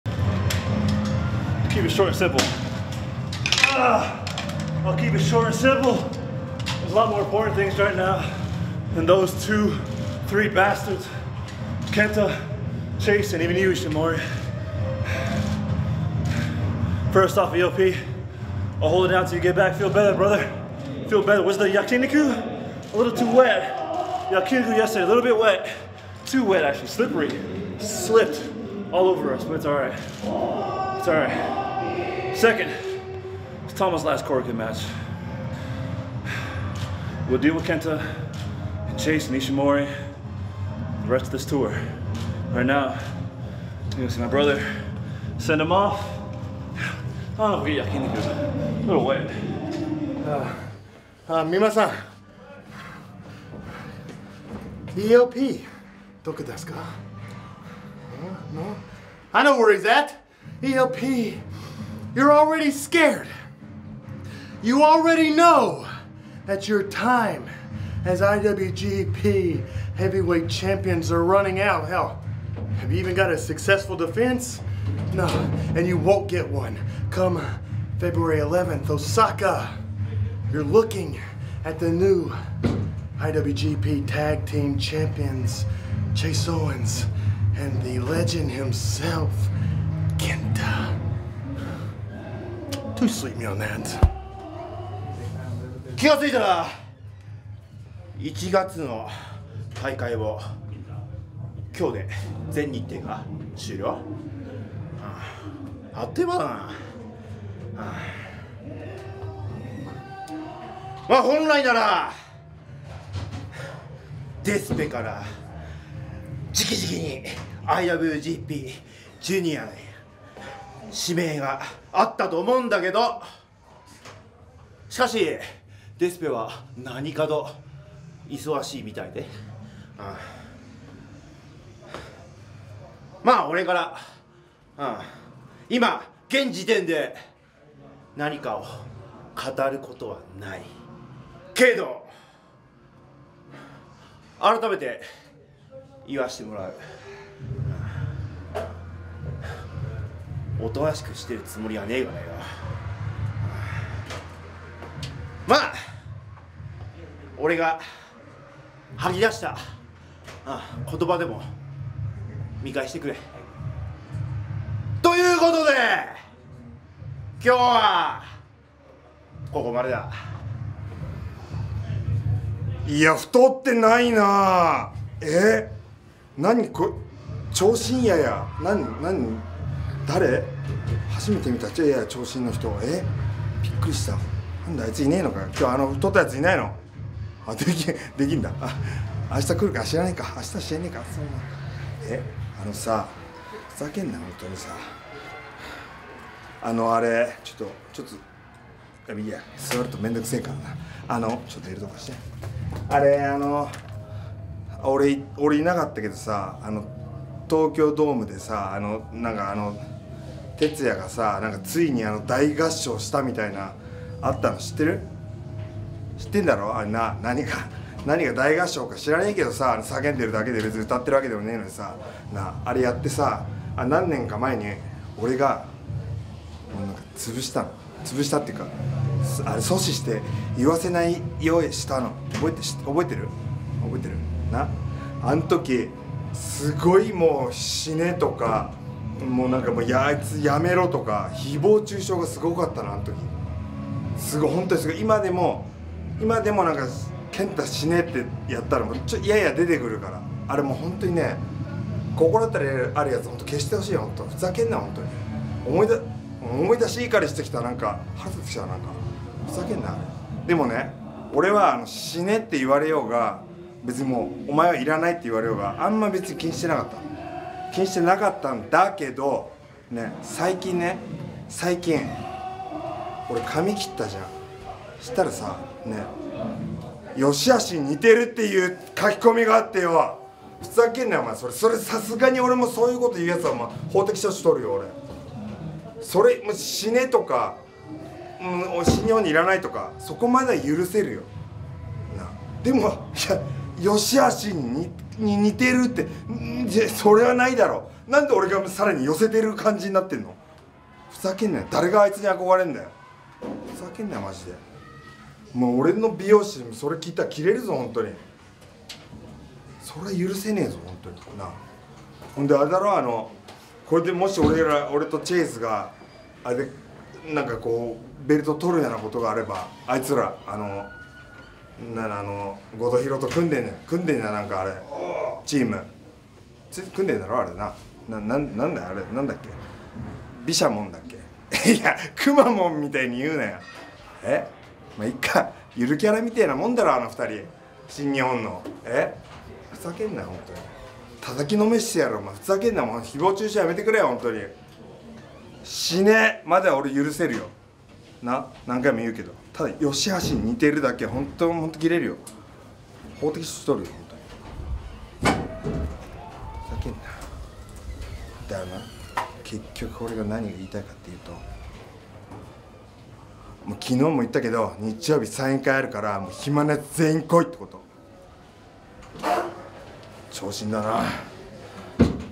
njnbg 2nd match Backstage 1/24/24｜Road to THE NEW BEGINNING 第2試合 Backstage
2024年1月24日(水) Road to THE NEW BEGINNING
第2試合：
田口隆祐 & 邪道 & ヒクレオ 🆚 石森太二 & チェーズ・オーエンズ & KENTA

January 24,2024 Road to THE NEW BEGINNING
Tokyo・Korakuen Hall
2nd match:
Ryusuke Taguchi , Jado and Hikuleo 🆚 Taiji Ishimori , Chase Owens and KENTA